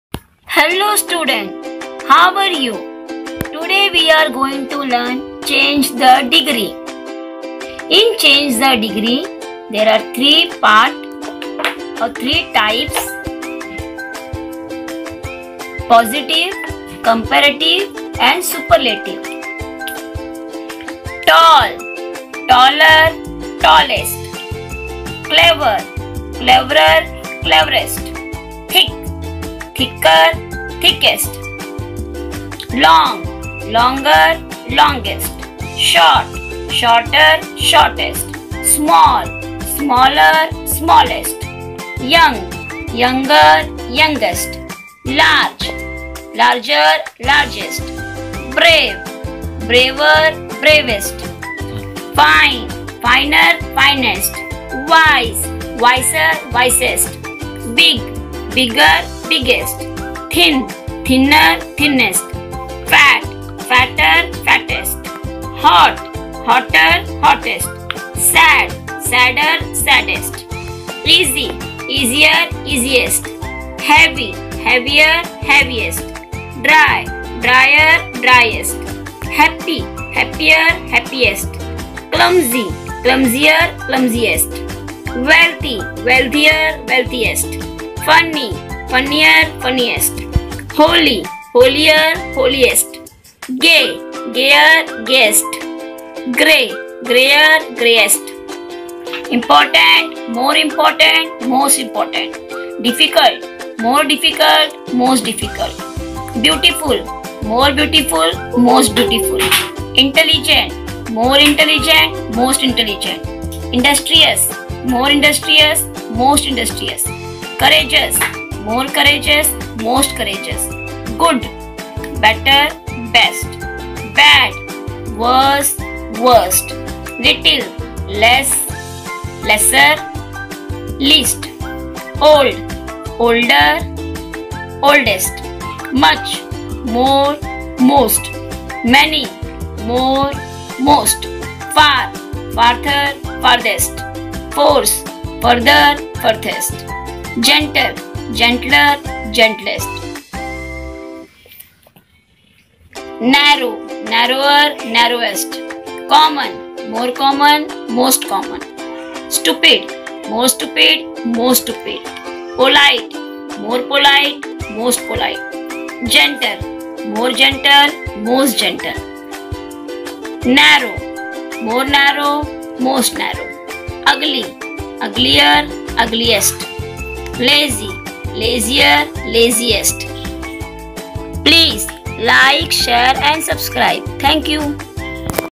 Change The Degree | Learn Degree Of Comparison In English Grammar | Comparison Of Adjectives
Welcome to the Kidz Educare Channel.

Voice: Farzana Shaikh,
Editing: KineMaster

Kidz Educare Team.